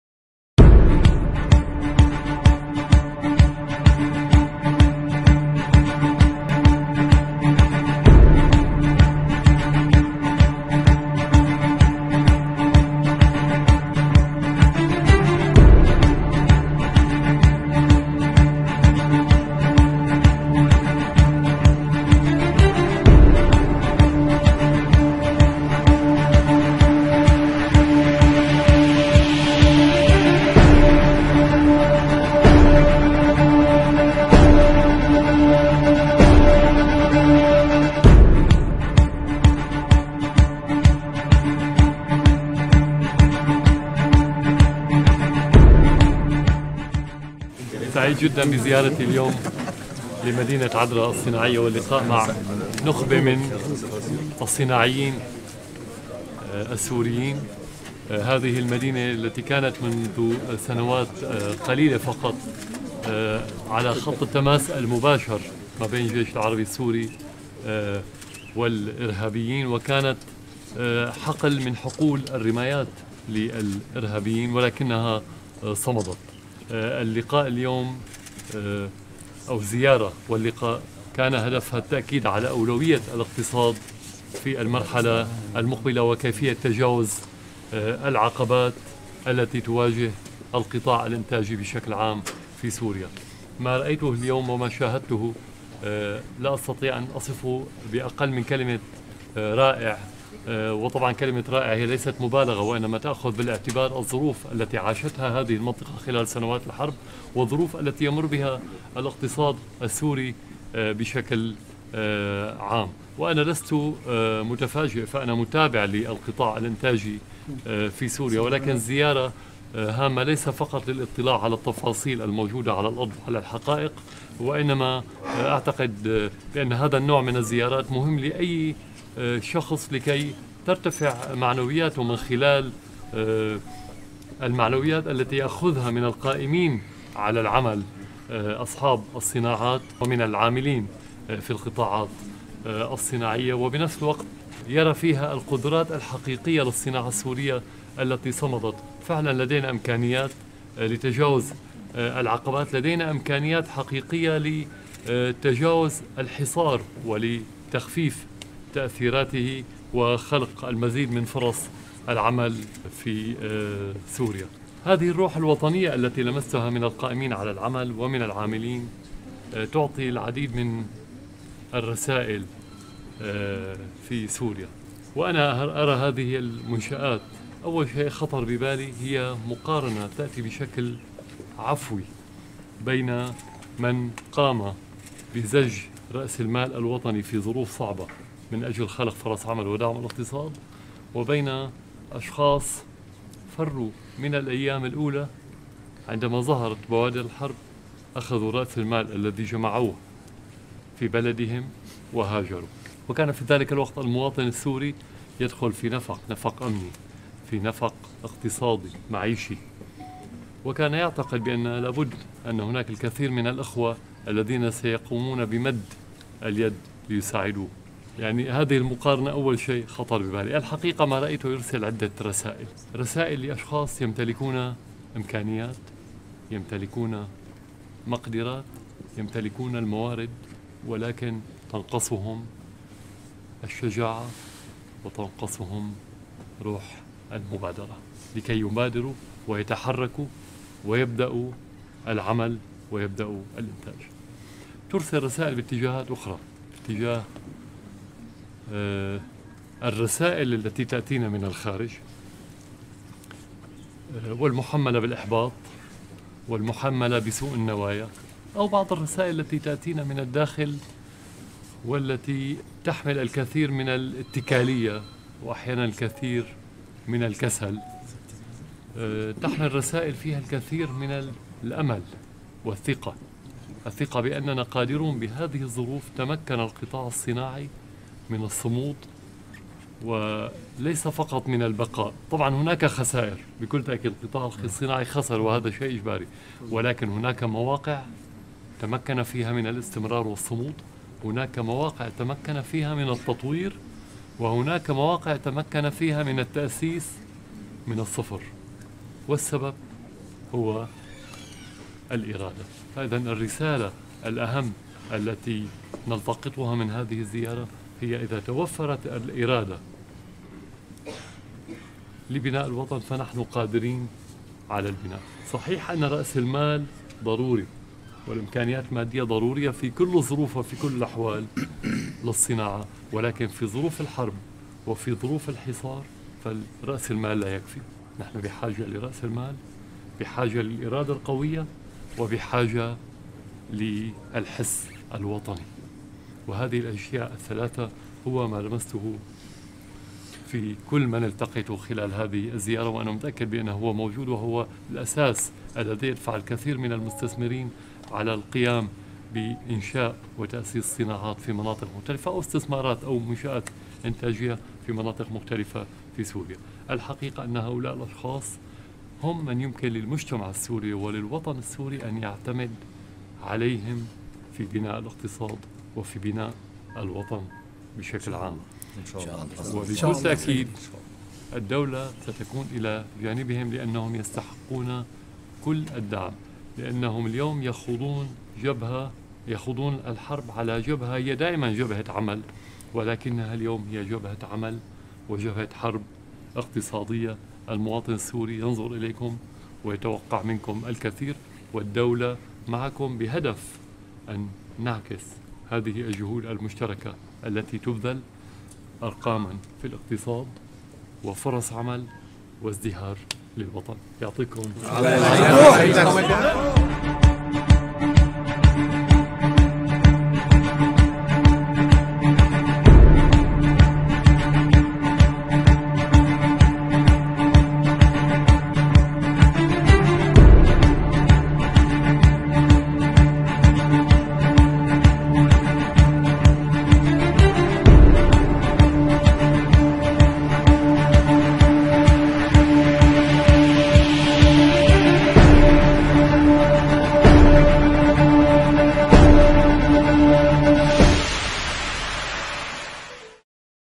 من حديث الرئيس الأسد مع الصناعيين وأصحاب المعامل والمنشآت في مدينة عدرا الصناعية
حديث الرئيس الأسد مع الصناعيين وأصحاب المعامل والمنشآت في مدينة عدرا الصناعية

432 مشاهدة

أعجبني

لم يعجبني

مشاركة

حفظ

الإبلاغ

Syrian Presidency

12.1 ألف مشترك

مشترك

تاريخ النشر: 09‏/06‏/2021

"إذا توفرت الإرادة لبناء الوطن فنحن قادرون على البناء، صحيح أن رأس المال والامكانيات المادية ضرورية للصناعة ولكن في ظروف الحرب والحصار نحن بحاجة لرأس المال وللإرادة القوية وللحس الوطني ... وهذا هو الأساس الذي يدفع الكثير من المستثمرين للقيام بإنشاء وتأسيس الصناعات في مناطق مختلفة في سورية، وبكل تأكيد الدولة ستكون إلى جانبهم لأنهم يستحقون كل الدعم فهم يخوضون الحرب على جبهة هي دائماً جبهة عمل ولكنها اليوم هي جبهة عمل وجبهة حرب اقتصادية". من حديث الرئيس الأسد مع الصناعيين وأصحاب المعامل والمنشآت في مدينة عدرا الصناعية اليوم